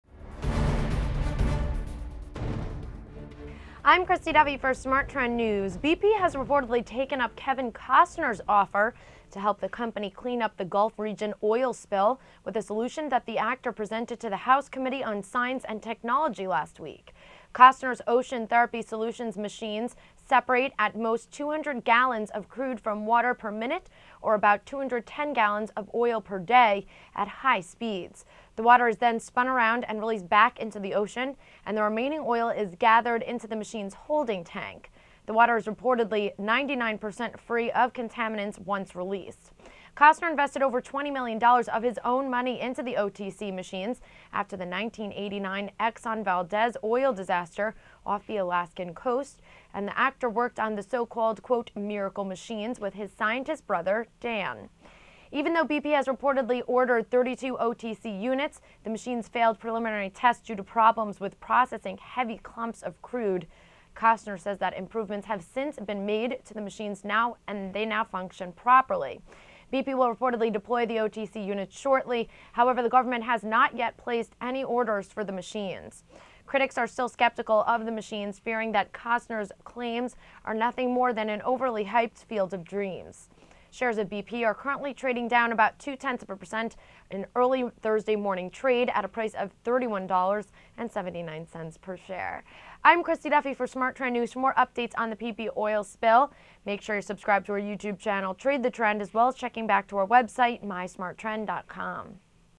News Update: BP Orders 32 of Kevin Costner's "Miracle Machines" to Clean up Gulf Region Oil Spill
BP Plc (NYSE:BP) has reportedly taken up Kevin Costner's offer to help the company clean up the Gulf region oil spill with a solution that the actor presented to the House Committee on Science and Technology last week.

Costner's Ocean Therapy Solutions (OTC) machines separate, at most, 200 gallons of crude from water per minute, or about 210 gallons of oil per day, at high speeds.

The water is then spun around and released back into the ocean, and the remaining oil is gathered into the machine's holding tank.

The water is reportedly 99 percent free of contaminants once released.

Costner invested over $20 million of his own money into the OTC machines after the 1989 Exxon (NYSE:XOM) Valdez oil disaster off the Alaskan coast, and the actor worked on the so-called "miracle machines" with his scientist brother, Dan.

Even though BP has reportedly ordered 32 OTC units, the machines failed preliminary tests due to problems with processing heavy clumps of crude; Costner says that improvements have since been made and the machines now function properly.

BP will reportedly deploy the OTC units shortly, however, the government has not yet placed any orders for the machines.

Critics are still skeptical of the machines, fearing that Costner's claims are nothing more than an overly-hyped field of dreams.

Shares of BP are trading down 0.19% Thursday morning at $31.79.